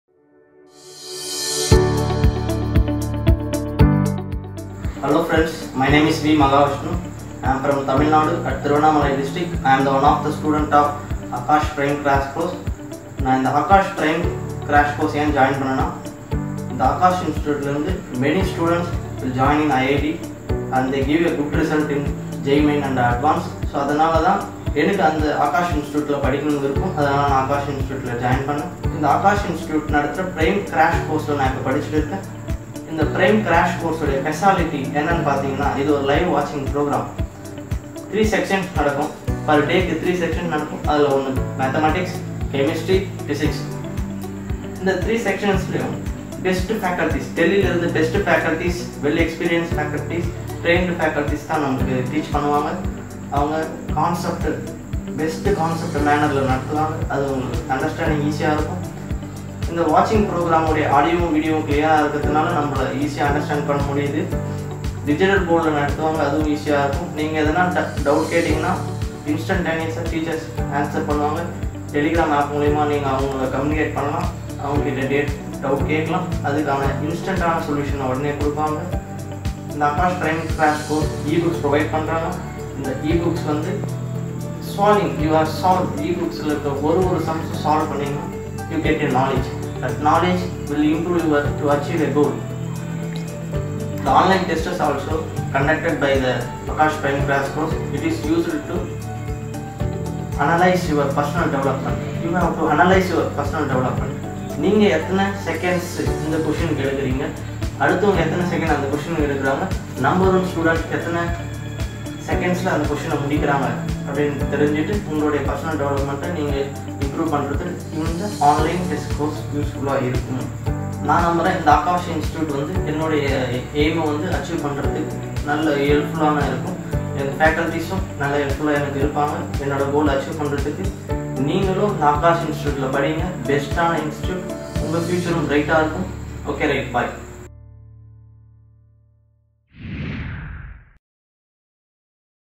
Watch What V. Maha Vishnu from Tamil Nadu has to say about Aakash PrimeClass
Aakash PrimeClass#Reinventing Education